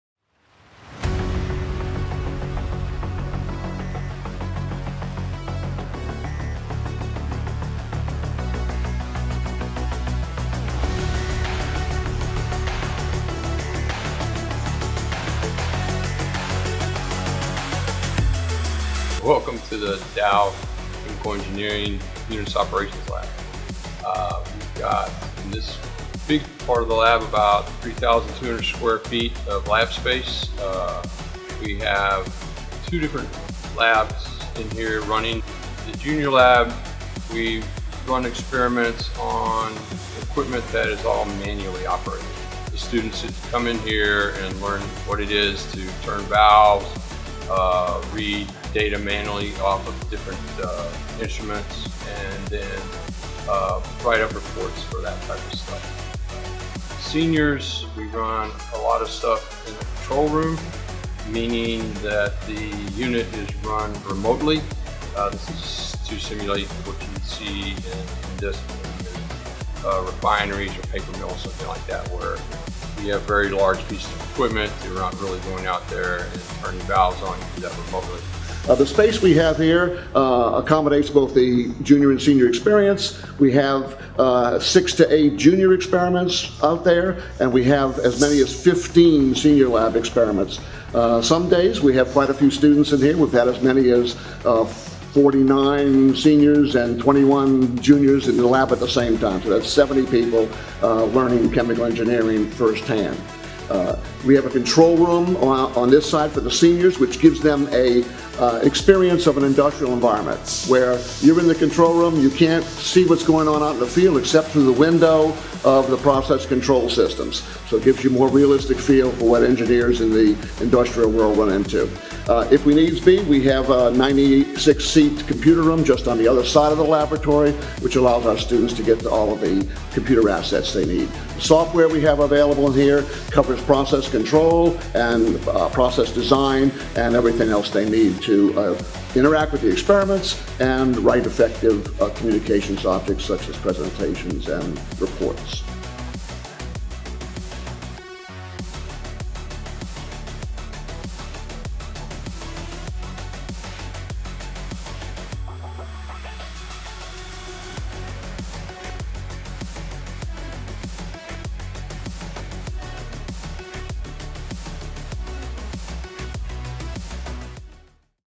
Dow Chemical Unit Operations Laboratory
Learn more about our Dow Chemical Unit Operations Lab and the work done inside by our Chemical Engineering students.